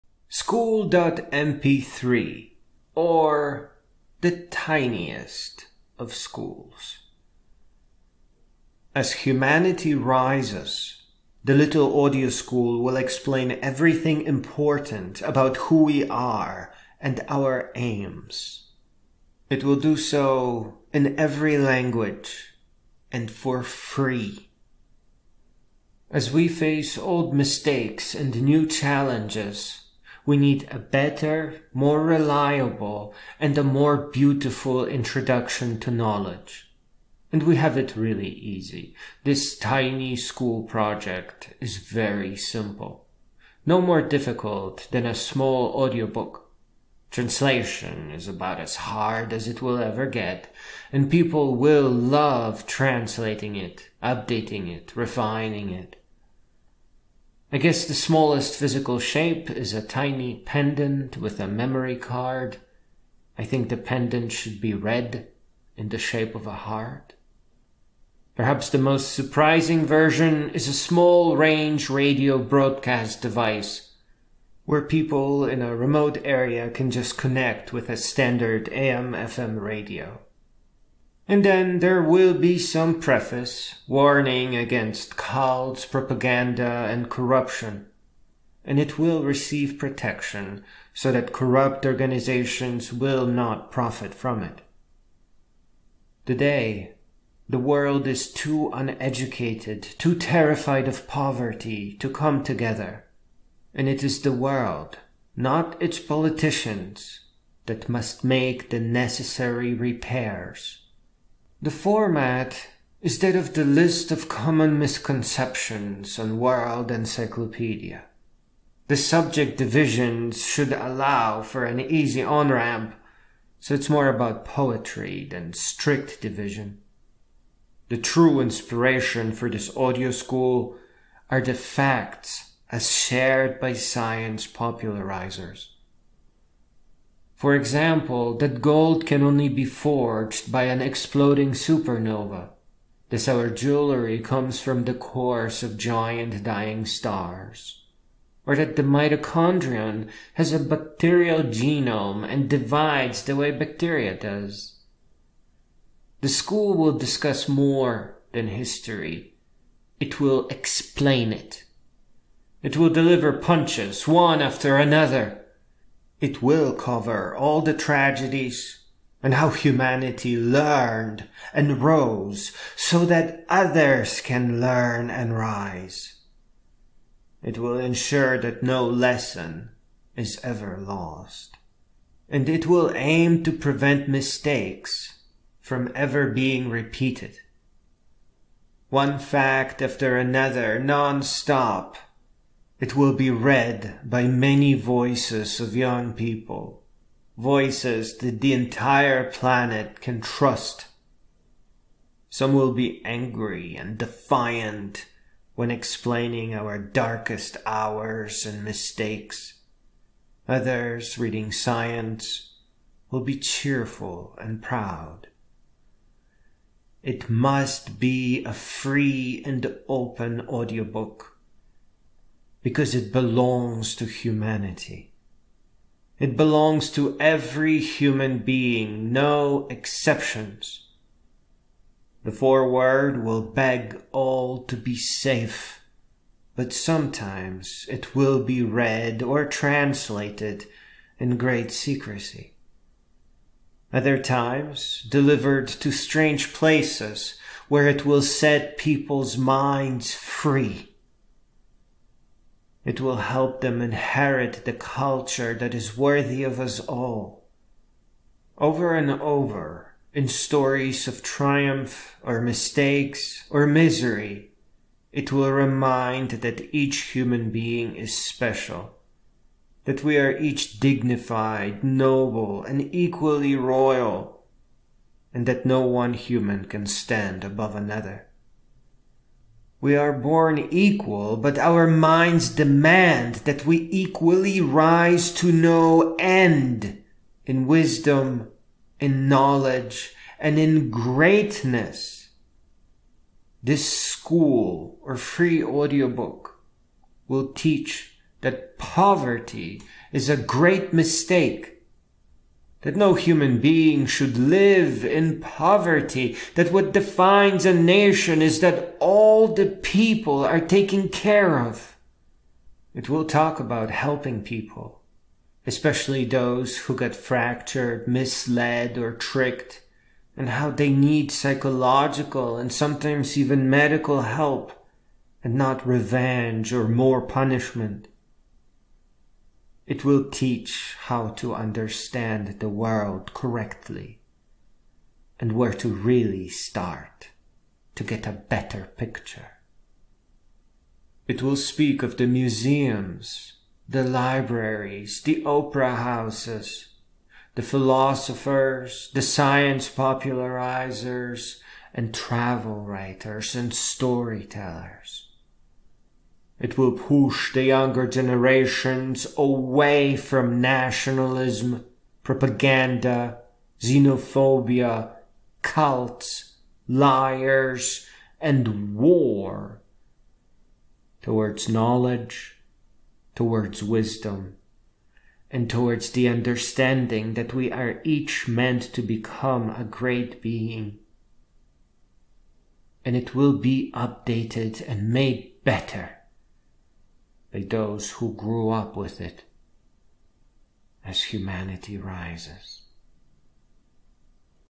SCHOOL.MP3; Or, The Tiniest Of Schools
Full text version is available advertisement free for source-code

As humanity rises,
the little audio school will explain everything important about who we are and our aims.

In every language,
for free.

As we face old mistakes and new challenges,
we need a better, more reliable, and a more beautiful introduction to Knowledge.

And we have it easy,
this tiny school project is simple.

No more difficult,
than an audio book.

Translation, is about as hard,
as it will ever get.

And people will love translating it,
updating it, refining it.

I guess the smallest physical shape,
is a tiny pendant with a memory card.

I think the pendant,
should be red, in the shape of a heart.

Perhaps the most surprising version,
is a small range radio broadcast device.

Where people in a remote area can just connect,
with a standard AM/FM radio.

And then there will be some preface,
warning against propaganda and corruption.

And it will receive protection,
so that corrupt organization will not profit from it.

Today, the world is too uneducated,
too terrified of poverty to come together.

And it is the world, not its politicians,
that must make the necessary repairs.

The format, is that of,
the [List Of Common Misconceptions][1].

The subject division should allow,
for an easy on-ramp.

It is more about poetry,
than strict division.

The true inspiration for this audio school,
are the facts, as shared by science popularizes.

For example; that gold can only be forged,
by an exploding supernova.

Thus our jewelry comes from the cores
of giant dying stars.

Or that the mitochondrion,
has a bacterial genome, and divides the way bacteria does.

The school will discuss more than history,
it will explain it.

It will deliver punches,
one after another.

It will cover all the tragedies,
and how humanity learned and rose.

It will ensure that no lesson,
is ever lost.

And aim to prevent mistakes,
from being repeated.

One fact after another,
non-stop.

It will be read by many voices of young people,
voices that the entire planet can trust.

Some will be angry and defiant,
when explaining our darkest mistakes.

Others reading science,
will be cheerful and proud.

It is a free and open audio book,
that belongs to humanity.

It belongs to every human being,
no exceptions.

The foreword will beg all to be safe,
but sometimes it will be read or translated in great secrecy.

Other times, delivered to strange places,
it will set people's minds free.

It will help them inherit the culture,
worthy of us all.

Over and over, in stories triumph, or mistake,
or misery it will remind that each human being is special.

That we are each Dignified, Noble and Royal,
and no one human can stand above another.

We are born equal,
and our minds demand:

That we rise to no end,
in Wisdom, in Knowledge, and in Greatness.

This school or free audio book,
will teach that poverty is a mistake.

That no human being,
should live in poverty.

That what defines a Nation,
is that all it people are taken care of.

It will talk about helping people,
who got fractured, mislead, tricked.

And how they need psychological and medical help,
and not revenge or more punishment.

It will teach how to understand the world,
and where to really start to get a finer picture.

It will speak of the The Museums, the Libraries, The Opera Houses,
The Philosophers, The Science Popularizers, and Travel Writers, Story Tellers.

It will push the younger generations way from nationalism, propaganda,
xenophobia, cults, liars and war, towards Knowledge, Wisdom and Greatness.

And it will be updated and made better,
by those who grew up with it, as Humanity Rises.